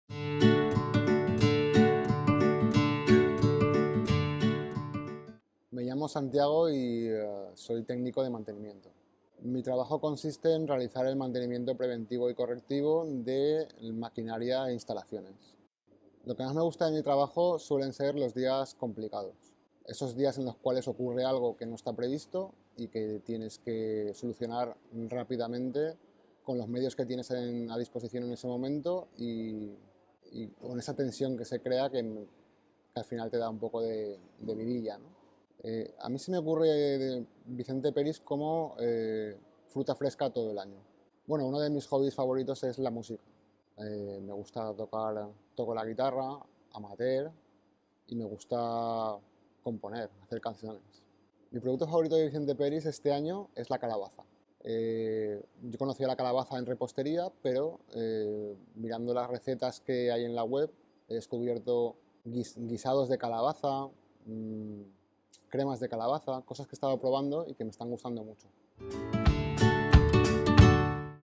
Equipo Peris: Santiago Montagud, técnico de mantenimiento
Os presentamos a Santiago Montagud, uno de nuestros técnicos de mantenimiento, el departamento que nos salva cada vez que alguna máquina hace cosas raras o se cansa de trabajar.

Le gustan los retos en su día a día laboral y trabajar con presión. En su tiempo libre la música le atrapa.

Él mismo os cuenta los detalles en este vídeo.